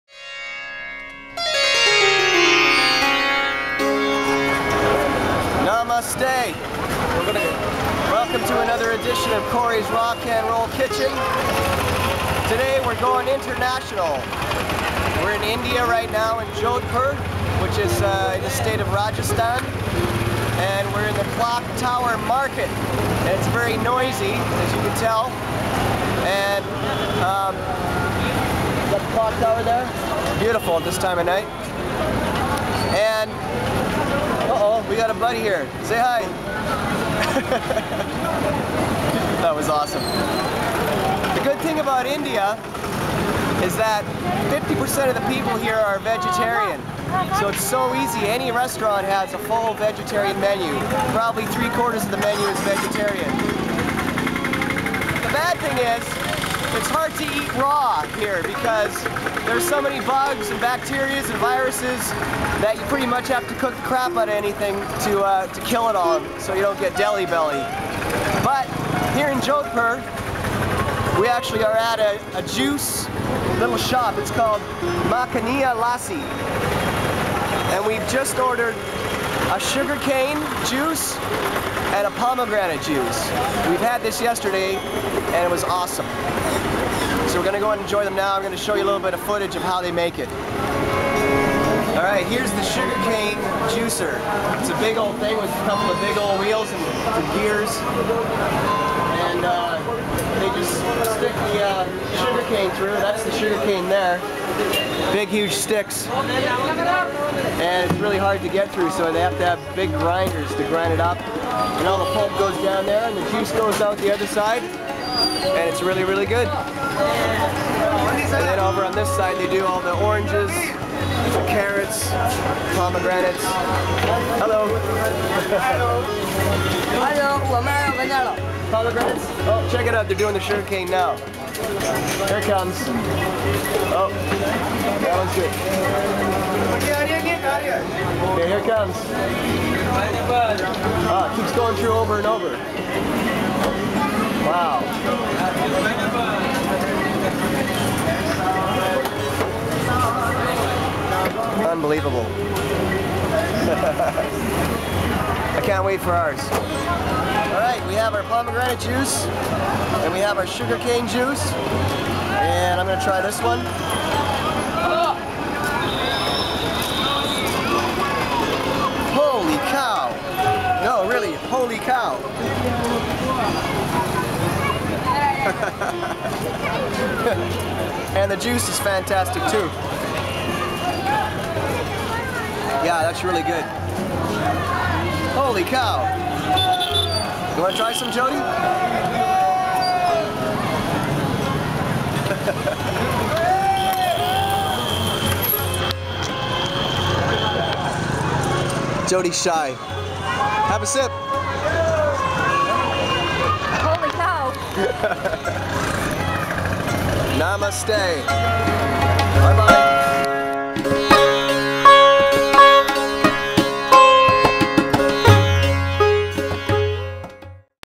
Raw Can Roll Kitchen - episode 11 - Jodhpur, India
Cory Churko shows you how they juice things like sugar cane in India.